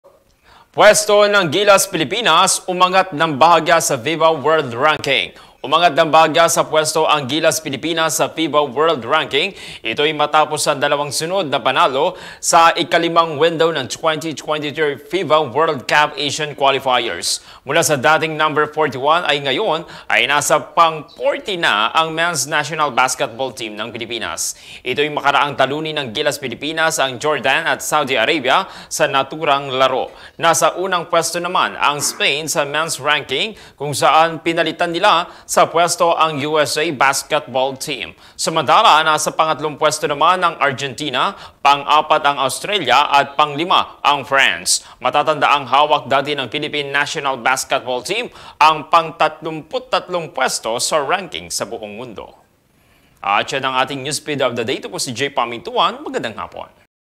Puwesto ng Gilas Pilipinas, umangat ng bahagya sa FIBA World Ranking
Umangat ng bahagya sa puwesto ang Gilas Pilipinas sa FIBA World Ranking, itoy matapos ang dalawang sunod na panalo sa ikalimang window ng 2023 FIBA World Cup Asian Qualifers.